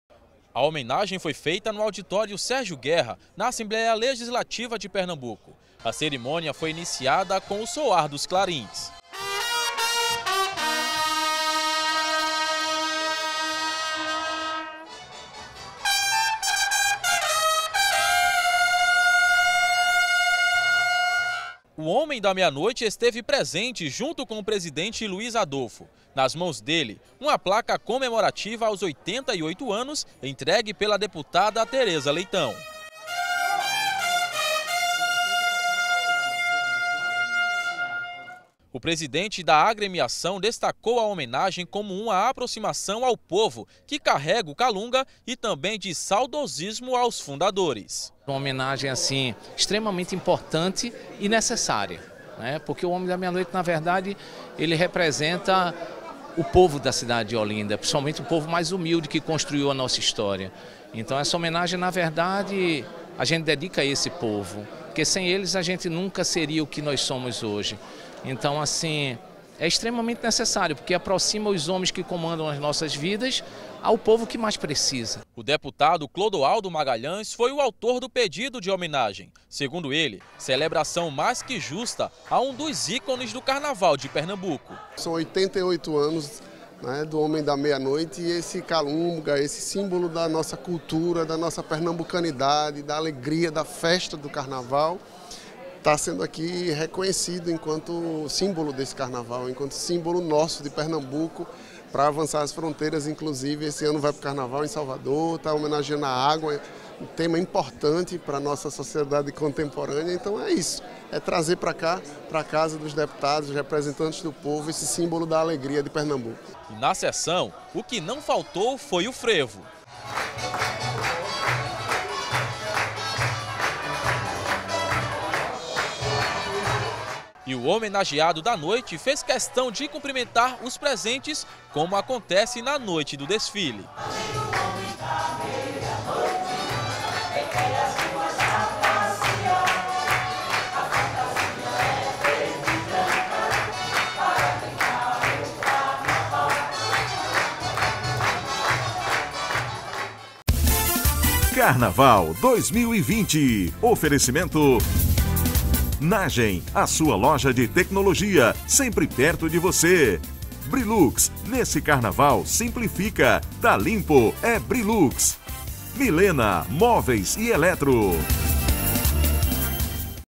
Homem da meia-noite é homenageado na Alepe
Homem da Meia-Noite é homenageado na Assembleia Legislativa de Pernambuco.